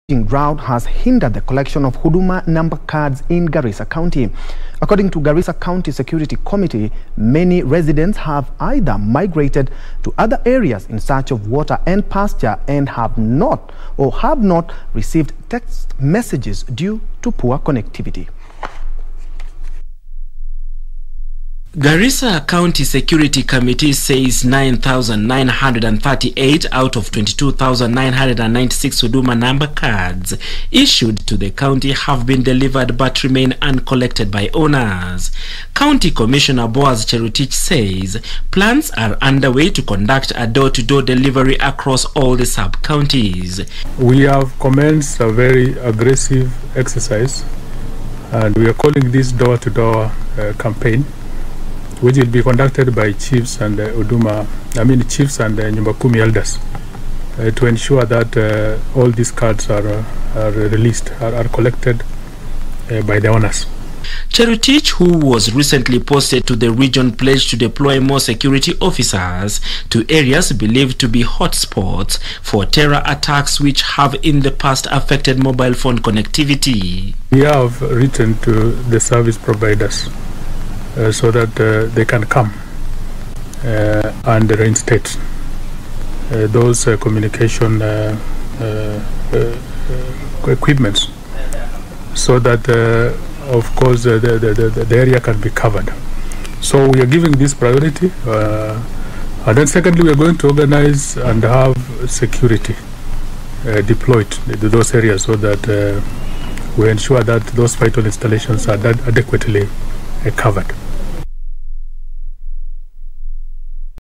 Lack of mobile phone network has hindered the collection of huduma namba cards in Garissa
Lack of mobile phone network and raging drought has hindered the collection of huduma namba cards in Garissa county. According to Garissa County Security Committee, many residents have either migrated to other areas in search of water and pasture or have not received text messages due to poor connectivity